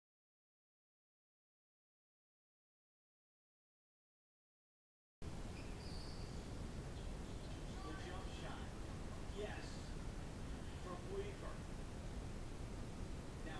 kyle weaver crossover
kyle weaver nasty crossover vs bobcats in summer league